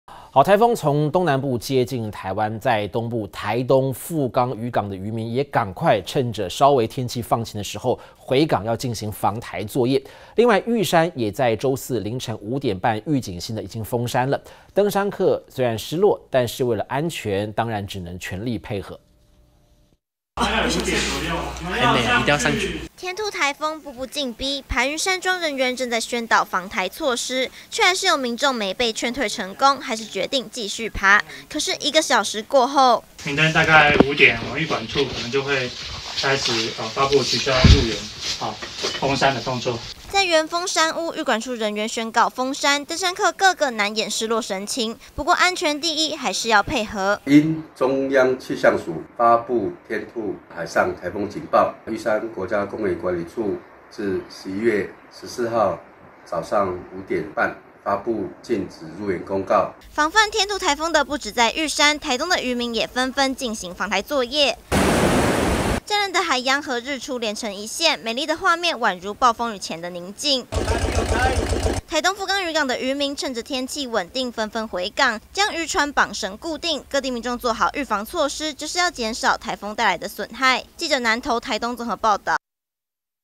防颱大作戰! 玉山周四清晨5:30起預警性封山 登山客遭勸退難掩失落 台東漁民回港繩綁船加固｜非凡財經新聞｜20241114
天兔颱風步步進逼，台東富岡漁港的漁民，趁著天氣放晴，回港進行防颱作業，另外，玉山，也在周四凌晨５點半，預警性封山，登山客雖然失落，但為了安全也只能全力配合。